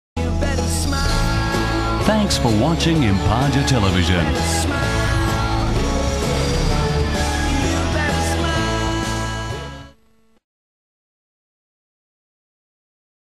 Imparja Ident 3A (2008)
Version 3A of the We-Heart-TV ident series from Imparja.

Video property of Imparja Television and Nine Network Australia.